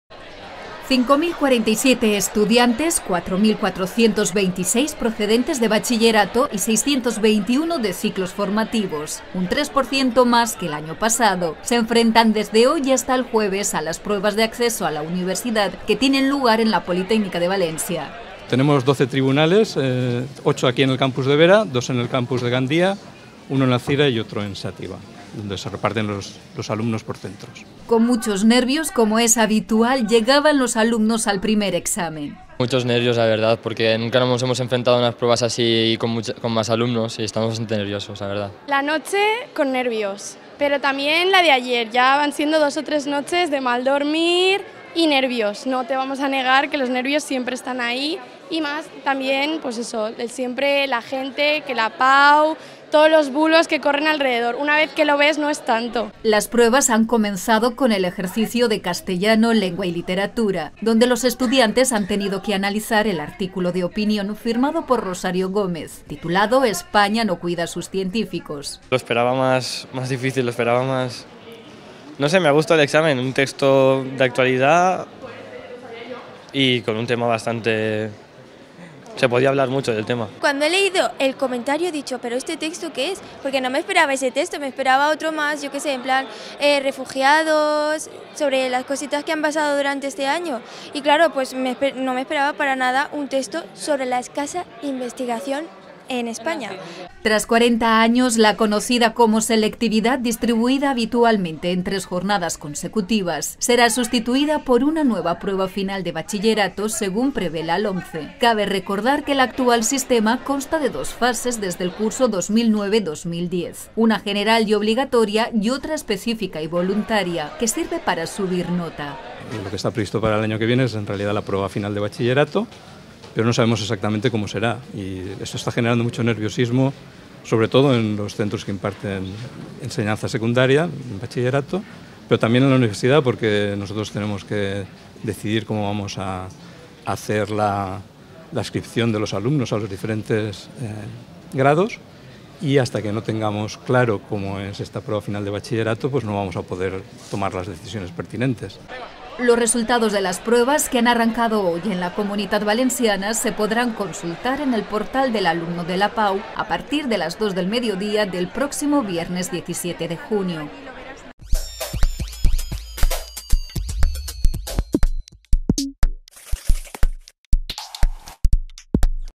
Pruebas de Acceso a la Universidad PAU 2016 - Noticia @UPVTV 07-06-2016
5.047 estudiantes – 4.426 procedentes de Bachillerato y 621 de Ciclos Formativos –, un tres por ciento más que el año pasado, se enfrentan desde hoy y hasta el jueves a las Pruebas de Acceso a la Universidad que tienen lugar en la Politècnica de València.

Con muchos nervios, como es habitual, llegaban los alumnos al primer examen. Las pruebas han comenzado con el ejercicio de Castellano: Lengua y Literatura, donde los estudiantes han tenido que analizar el artículo de opinión firmado por Rosario G. Gómez, titulado “España no cuida a sus científicos”.

Tras 40 años, la conocida como Selectividad – distribuida habitualmente en tres jornadas consecutivas – será sustituida por una nueva prueba final de Bachillerato, según prevé la LOMCE. Cabe recordar que, el actual sistema consta de dos fases desde el curso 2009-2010: una general obligatoria y otra específica y voluntaria, que sirve para subir nota.

Los resultados de las pruebas que han arrancado hoy en la Comunitat Valenciana se podrán consultar en el portal del alumno de la PAU a partir de las dos del mediodía del próximo viernes, 17 de junio.